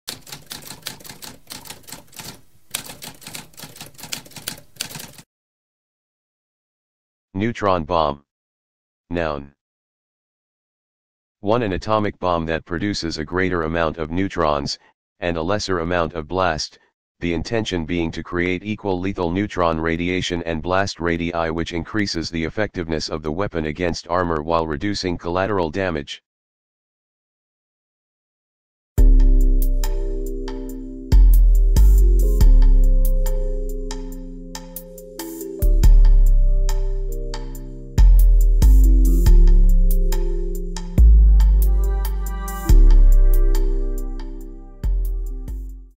What does neutron bomb mean?
A spoken definition of neutron bomb.

Intro Sound:
Typewriter - Tamskp